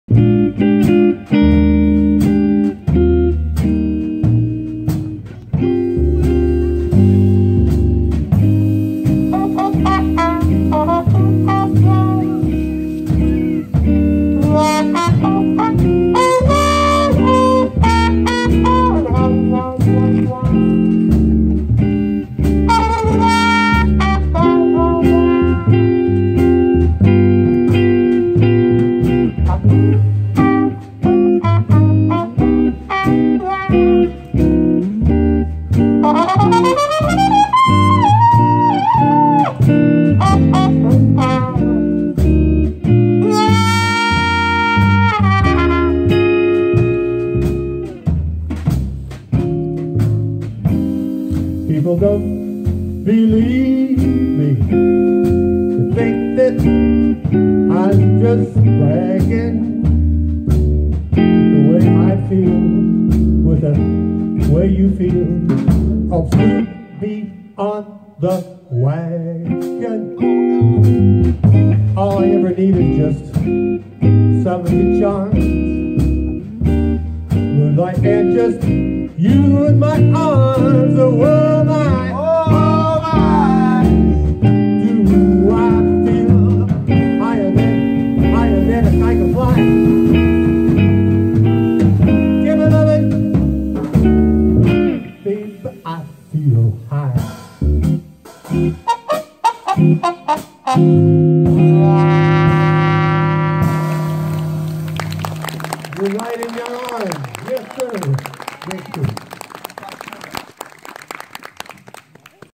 Scotch and Soda final
It was such a pleasure to play for the good people of Lac Wentworth to  finish off the summer , We hope to come back nxt year